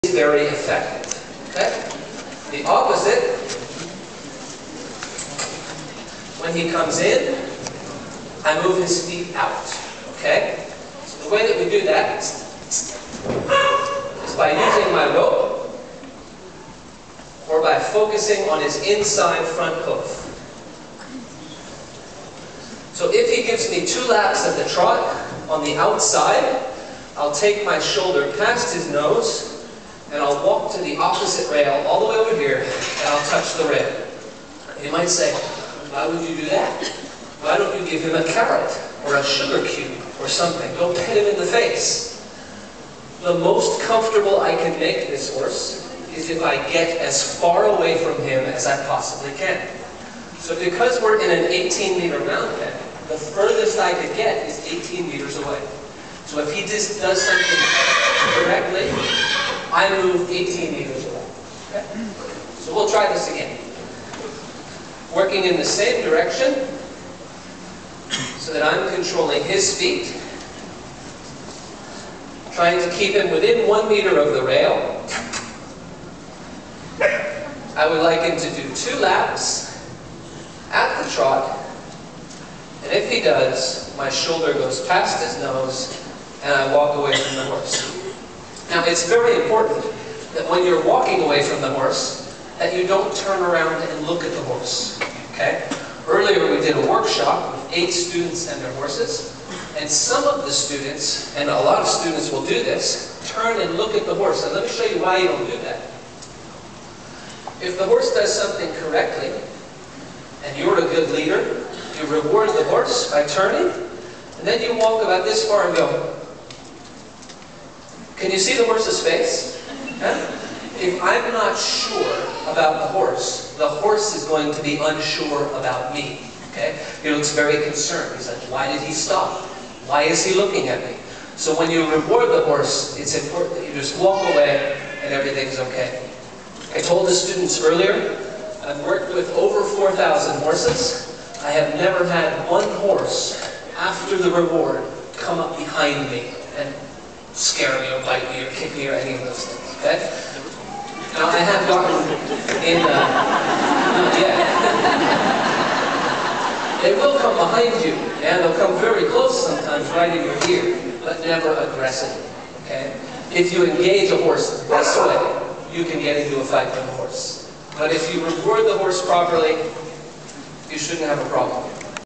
GaWaNi Pony Boy
GaWaNi Ponyboy@Natural Horsemanship day 2008, Arnhem, The Netherlands